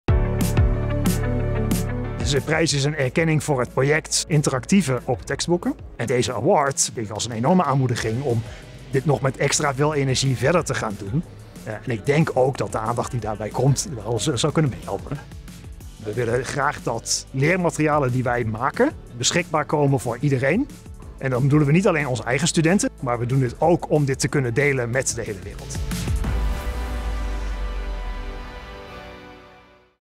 Timon Idema wint een SURF Onderwijsaward
Timon was een van de eersten die het belang inzag van open leermaterialen, en dit ook in de praktijk bracht. In 2018 publiceerde hij zijn eerste open textbook, voor de opleiding Nanobiology (TU Delft & Erasmus Universiteit). Met hulp van een bijdrage door de stimuleringsregeling van SURF heeft Timon daarna, met hulp van collega’s, een reeks open leermaterialen ontwikkeld voor de bachelor- en masteropleiding Nanobiology.